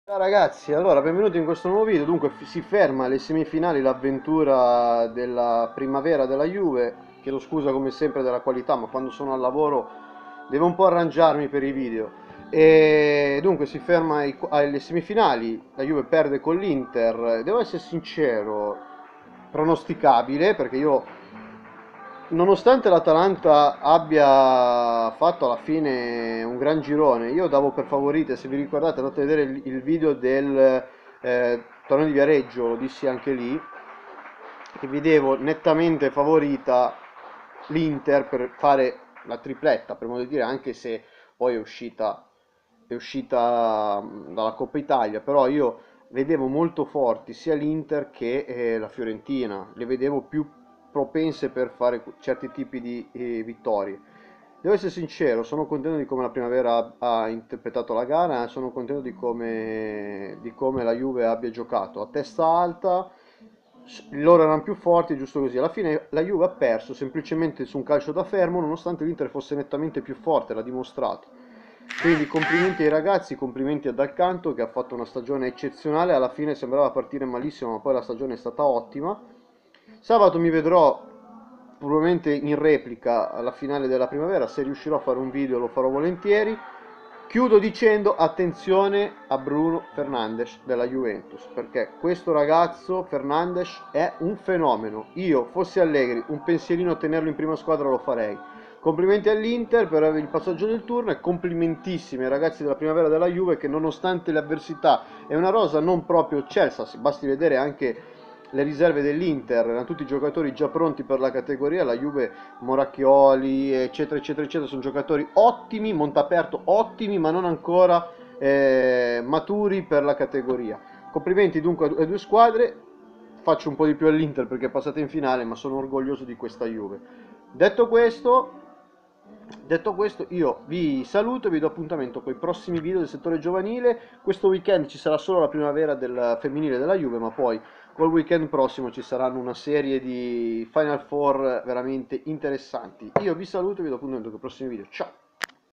Inter - Juve 1-0 Primavera 1 Semifinale
si ferma qui l avventura dei ragazzi di Dal Canto ORGOGLIOSO DI VOI